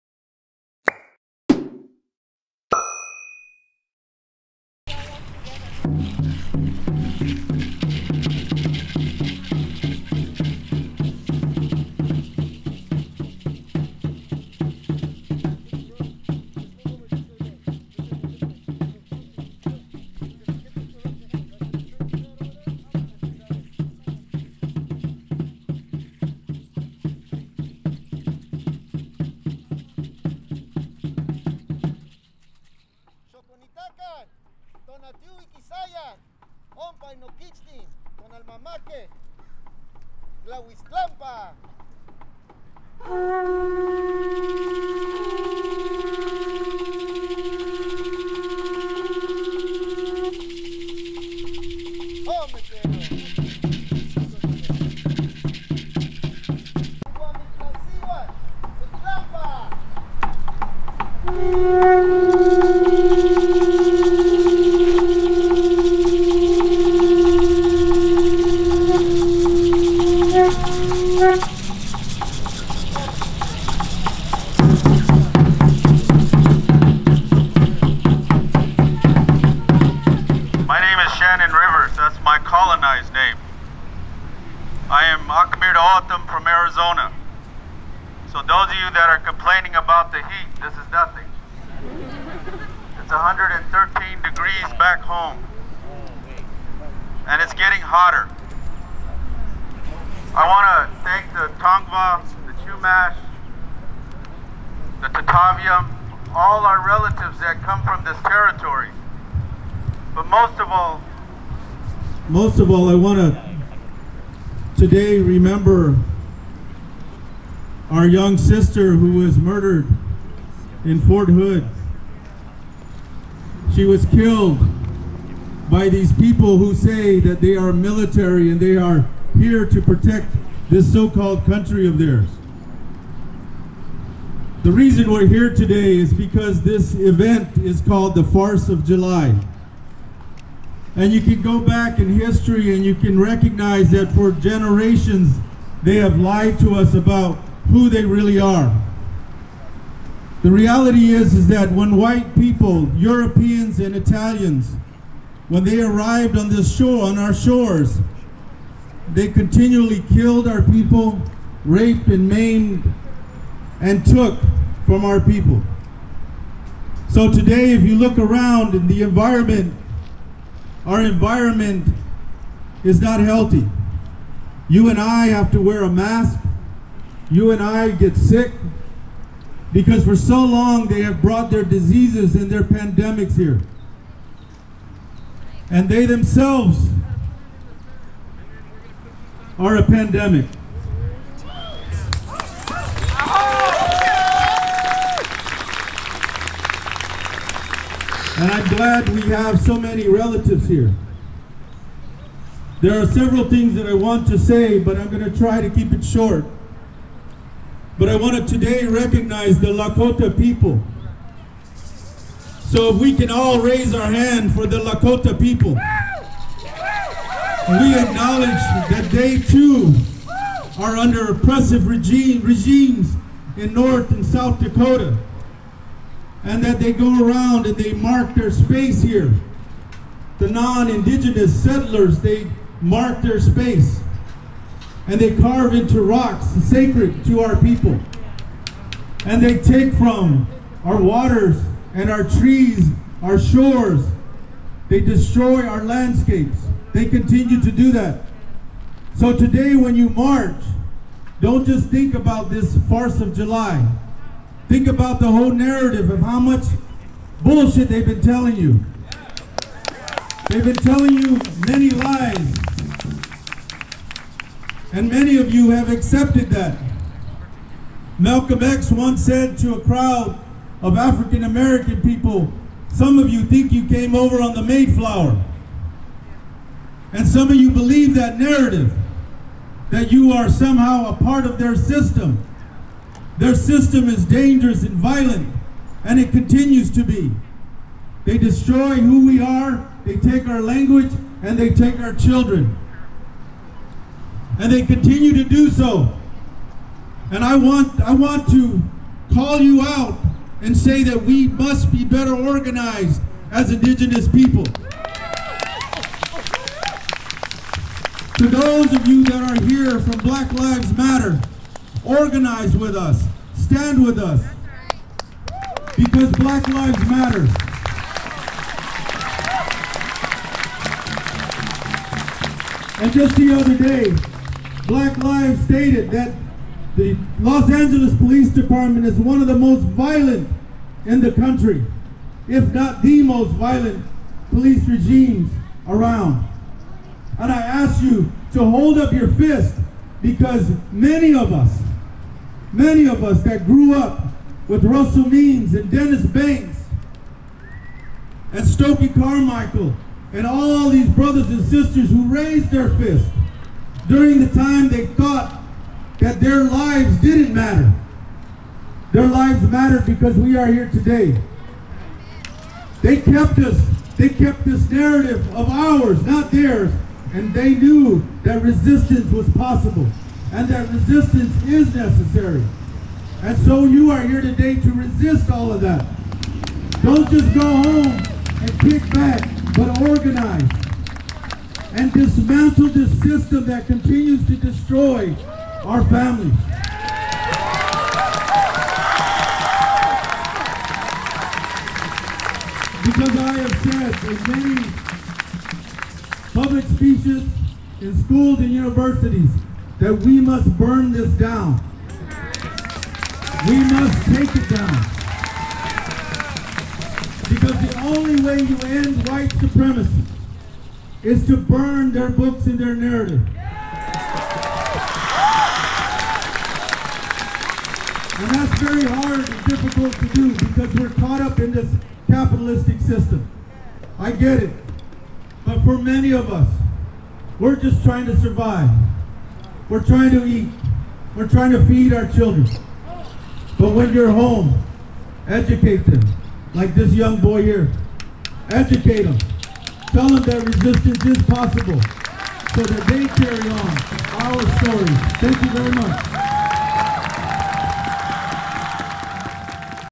The Farce of July March on Survivors Day / It is totally different event on 4th of July
The inception of the Farce of July began in 1997 in Los Angeles in order to raise funds for grassroots organizations that advocate for human rights and the environment. Unlike many 4th of July events, the Farce of July is strictly no alcohol, drugs, or fireworks.